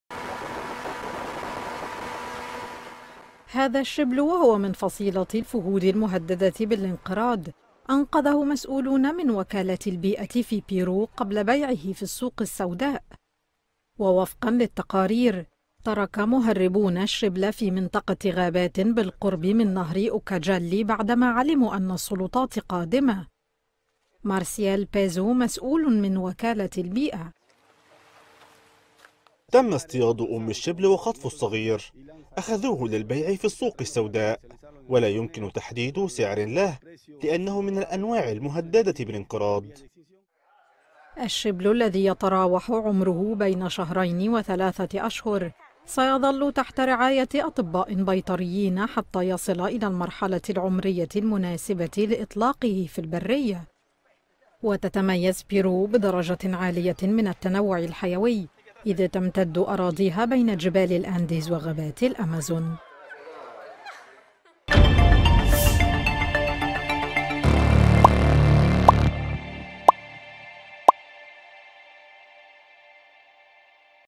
بيرو تنقذ شبل فهد مهدد بالانقراض قبل بيعه في السوق السوداء
أنقذ مسؤولون من وكالة البيئة في بيرو شبل فهد وهو نوع مهدد بالانقراض كان على وشك أن يباع في السوق السوداء.